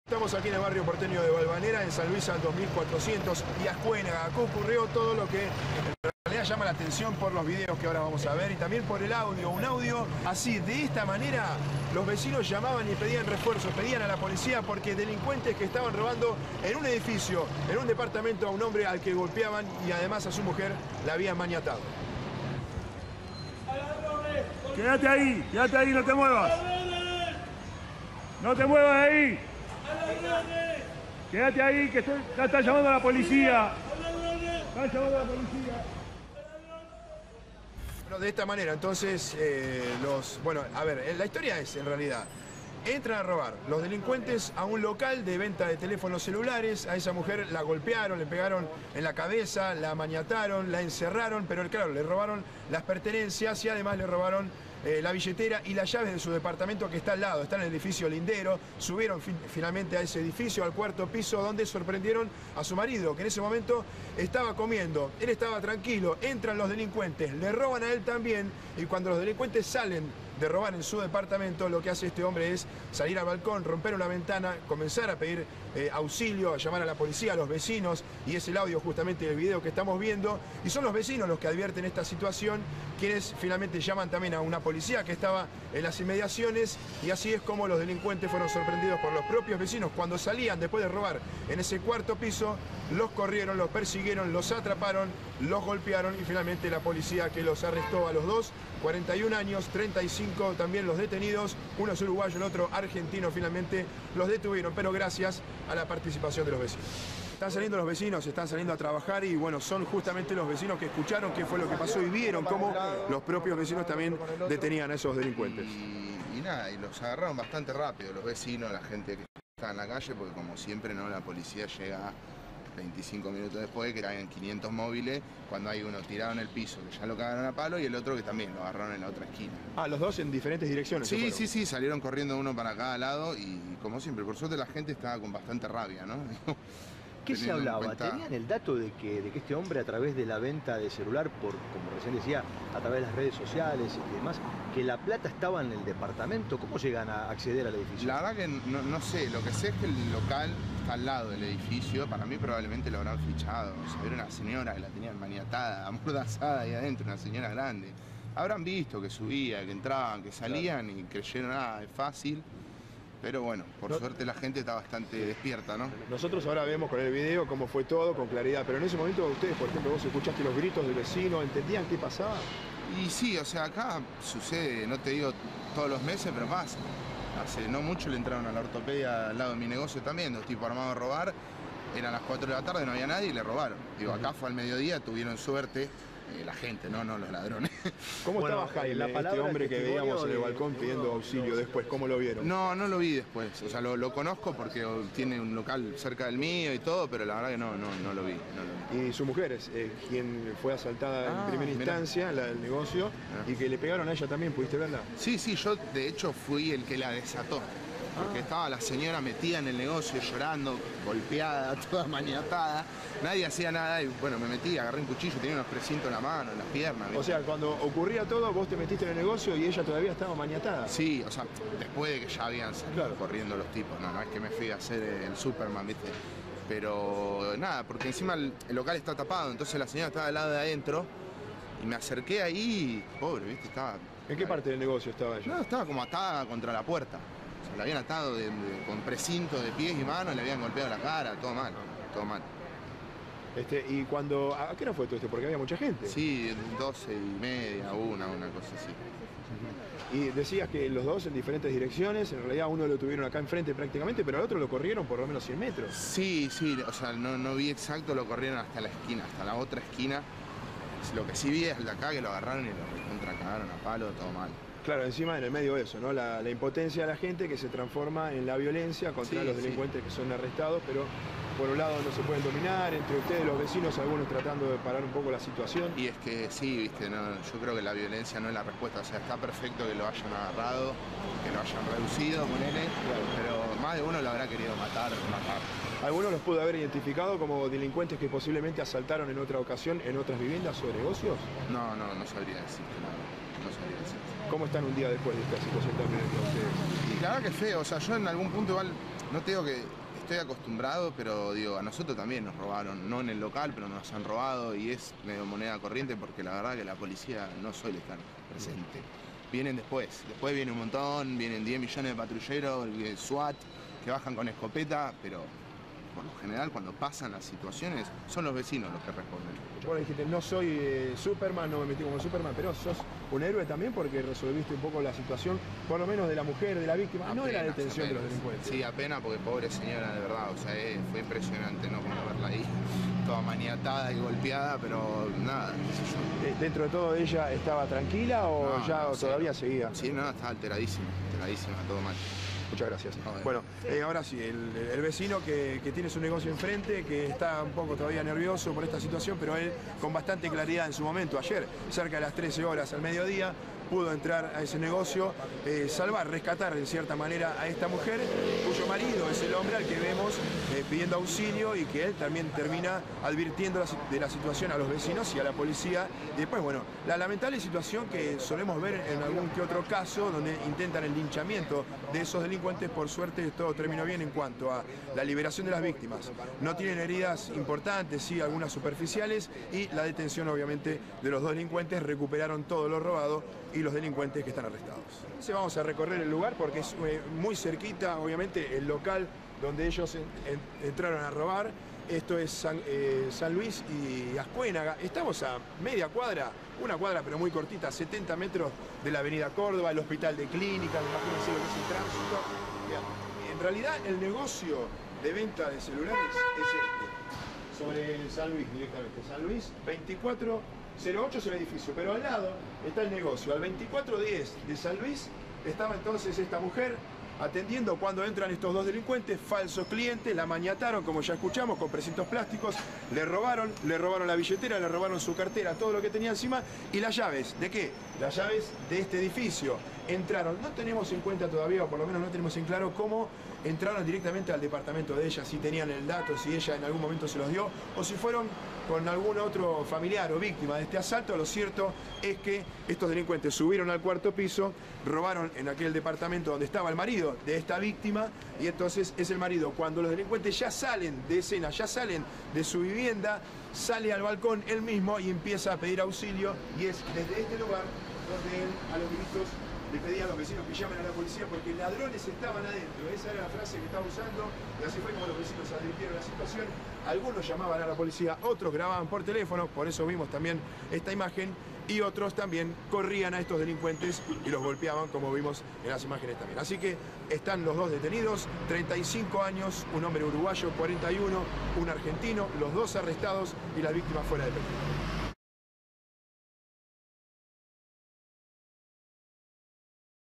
Desde el balcón: entraron a robar y salió al balcón a pedir ayuda. Lo ayudaron los vecinos
Delincuentes asaltaron a un hombre en su casa. Acorralado, salió al balcón y empezó a gritar para pedirles ayuda a los vecinos. Funcionó:  los delincuentes se escaparon y los vecinos alertaron a la Comisaría Vecinal 3 A de la Policía de la Ciudad. Cuando llegaron, con la ayuda de los propios transeúntes lograron detener a uno de los ladrones. Lo esposaron a un poste de luz. Los vecinos le pegaron patadas y trompadas mientras llegaban más refuerzos policiales.Mientras tanto, oficiales de la Fuerza Delta de la policía porteña, detuvieron al segundo delincuente en Azcuénaga y San Luis. Se trata de un ciudadano de nacionalidad uruguaya de 35 años y un argentino de 41. Sebastián Domenech, con toda la secuencia del asalto.